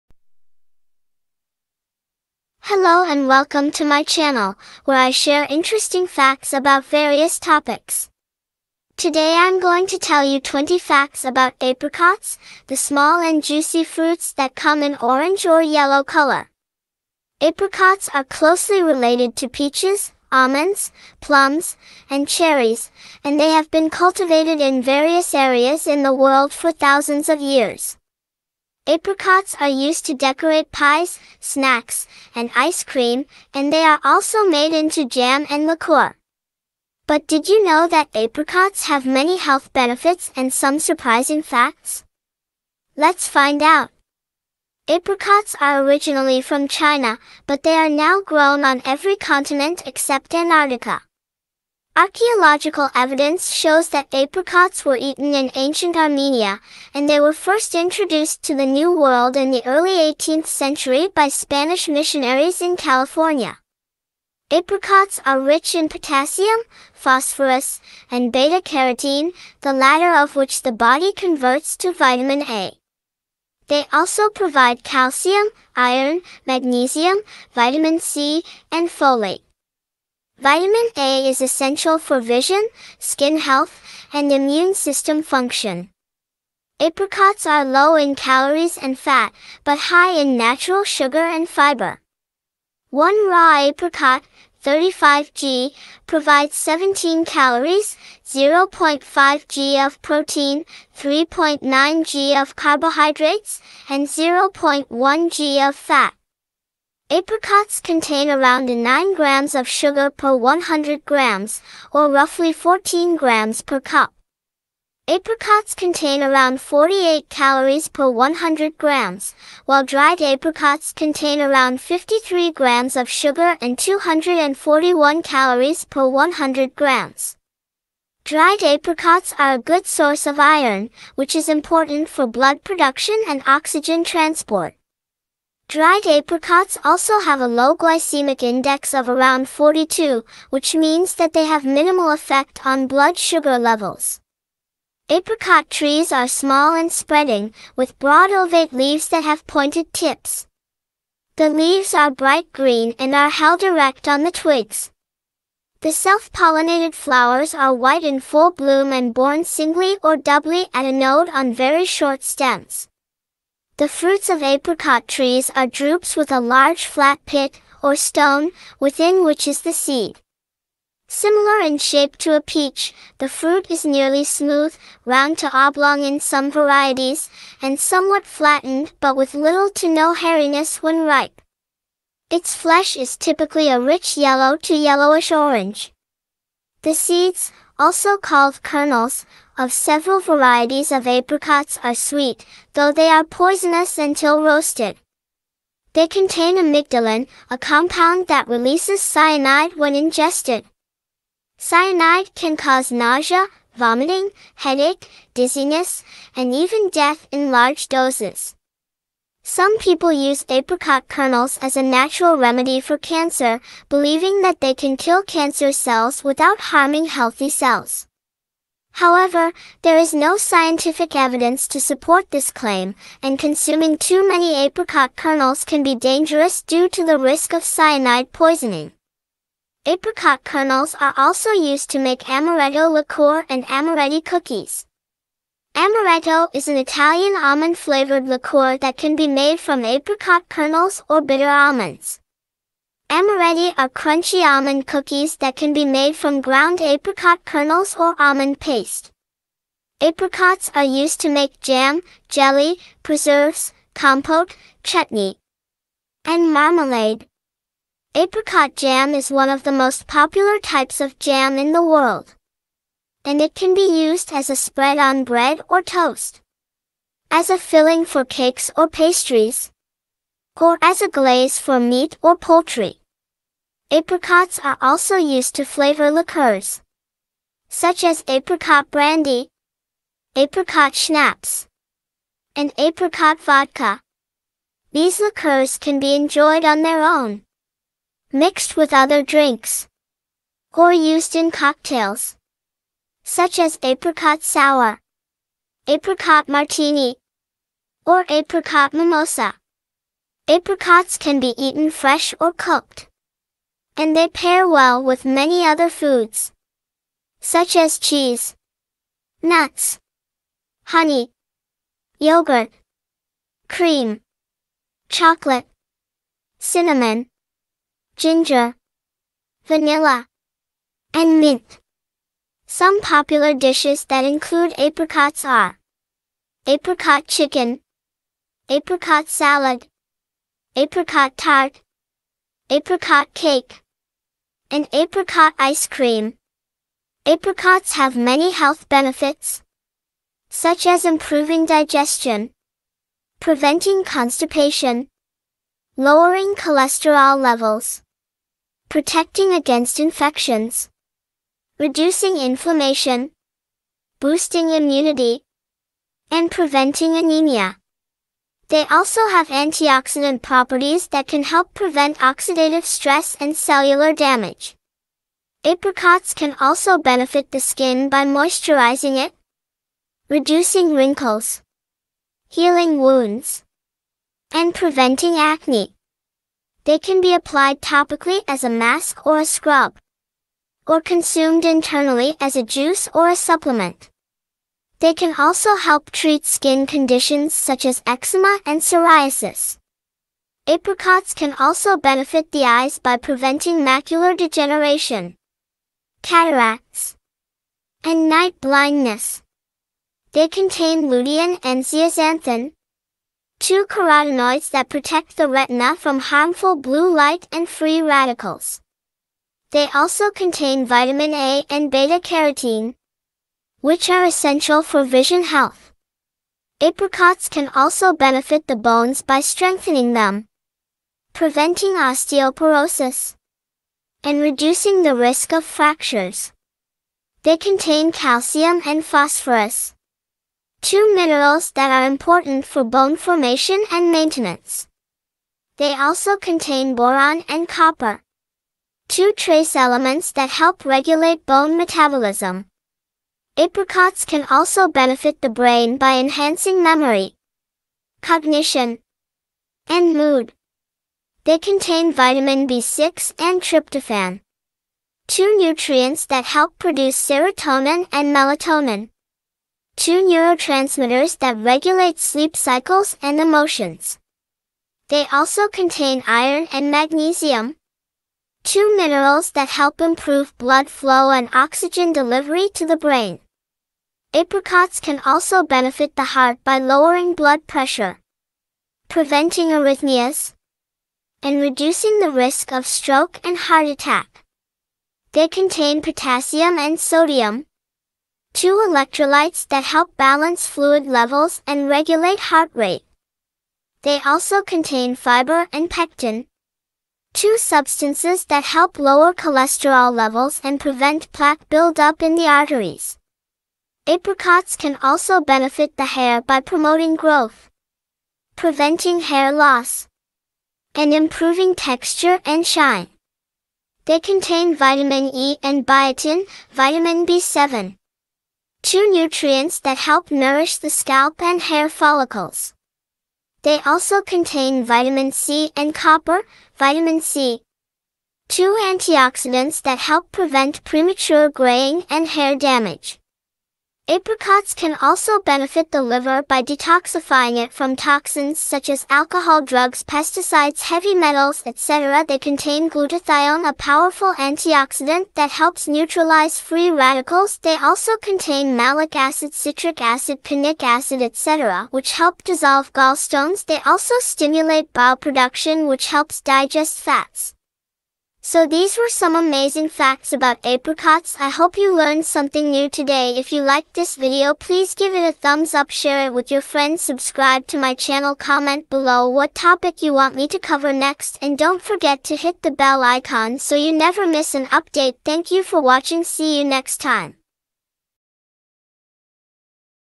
How Apricots Can Benefit Your Health Top 20 Facts You Should Know
In this video, you will learn 20 fascinating facts about apricots, the sweet and juicy fruits that come in different colors and shapes. You will discover where they come from, how they are used, and what benefits they have for your health, skin, eyes, bones, brain, heart, hair, and liver. You will also find out some surprising facts about their seeds, flowers, and trees. By the end of this video, you will have a new appreciation for apricots and maybe even want to try some yourself.